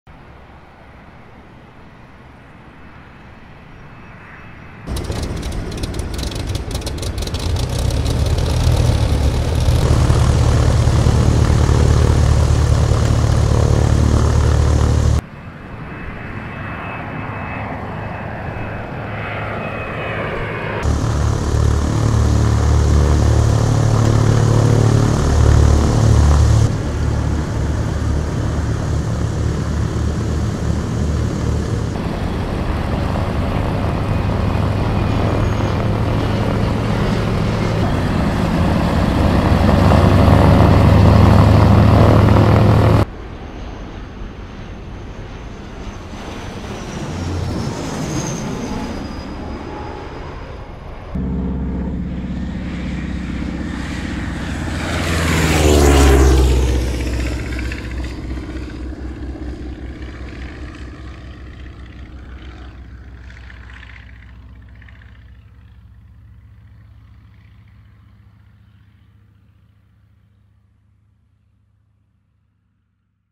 Real Lego airplane at the airport - Made with Blender
Real Lego Technic 42025 cargo plane in the airport.
Lego Airplane with twin propeller engine in the real world!

3D animation made with Blender

for the drone video shooting

for the 3d cad model